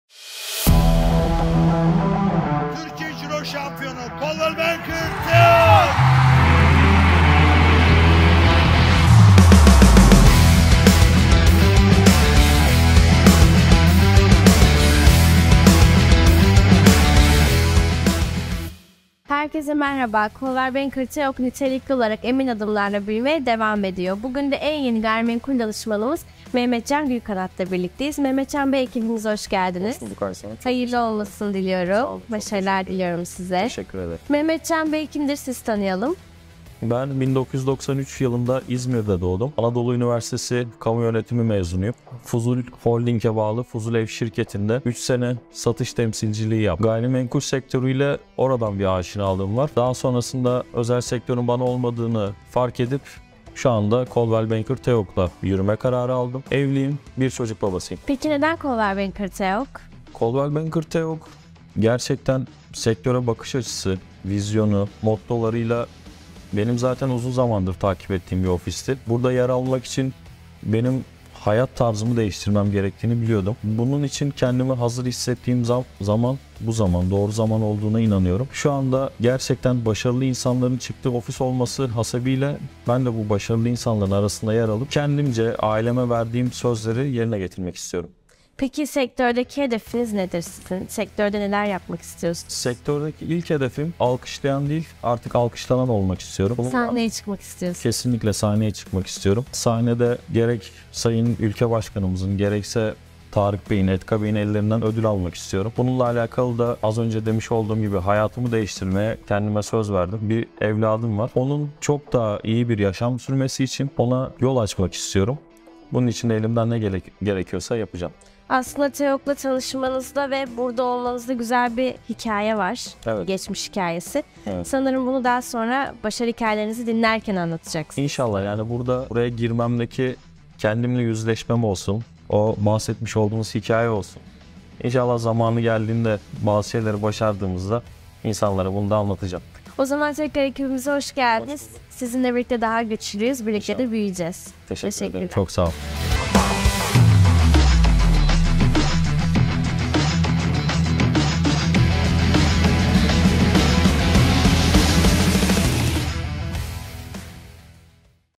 TEOK TAKIMI BÜYÜMEYE DEVAM EDİYOR ! I MEHMETCAN GÜLKANAT
✨ CB Teok Nitelikli Olarak Büyümeye Devam Ediyor ! ✨

3 sene gayrimenkul satış sektöründe çalışan Mehmetcan Gülkanat, kariyerini özel sektörün sınırlandırması değil, kendi vizyon ve emekleriyle yükseltmek istiyor. Başarılarını Teok Gayrimenkul çatısı altında duyurmak isteyen en yeni profesyonelimiz Mehmetcan  Bey'i tebrik ediyor, takımımıza hoş geldiniz diyoruz 👏🏻💫